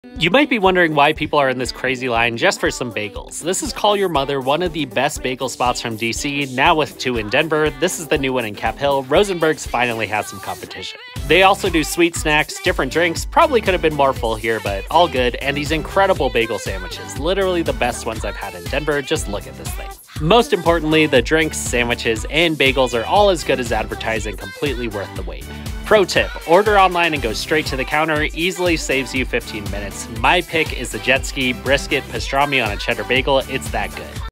The New Best Bagel in Denver? 🥯
Call Your Mother has arrived in Denver with two locations (Tennyson and Cap Hill), and these bagels are as good as advertised. While Rosenberg's has been the bagel champion of Denver for forever, there's finally some bagel competition in the city from one of Washington DC's best bagel shops. With some of the best bagels and bagel sandwiches in Denver, Call Your Mother is definitely worth a visit when you're in the city.